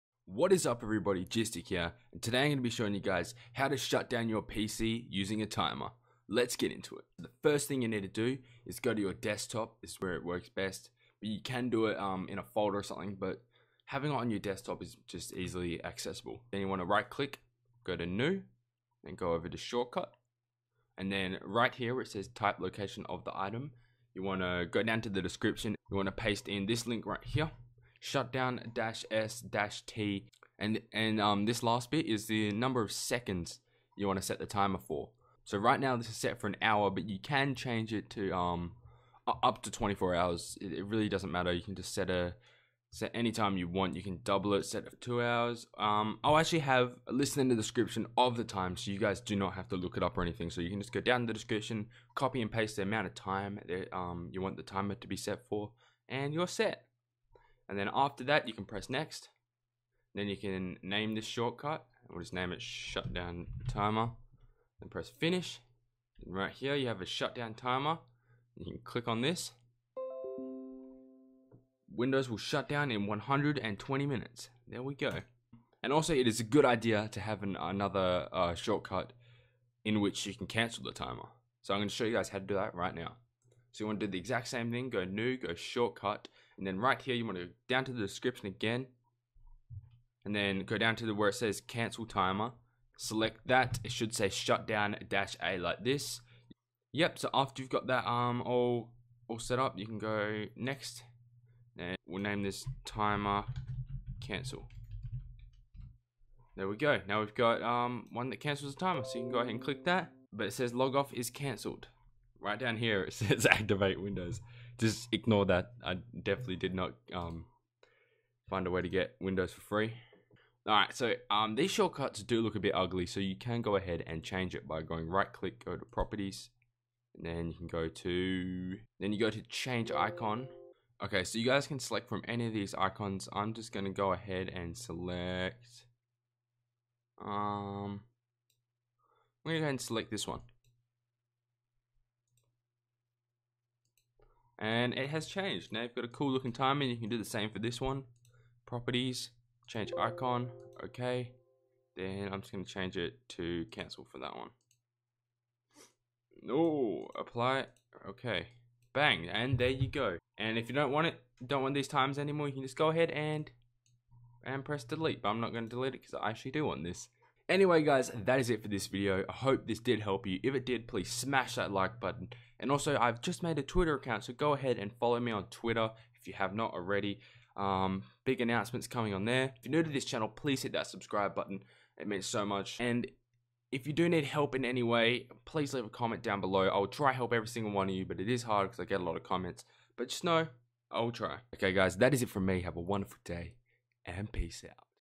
How To Shutdown Your PC Automatically Using A Timer (Windows 10)
Shutdown Timer: shutdown -s -t 3600
Cancel Timer: shutdown -a

Time for shutdown command:
1min=60sec
5min=300sec
10min=600sec
15min=900sec
20min=1200sec
30min=1800sec
45min=2700sec
1h=3600sec
2h=7200sec
3h=10800sec
5h=18000sec
10h=36000sec
24h=86400sec

Thankyou for all of your support!
~ GysticTutorials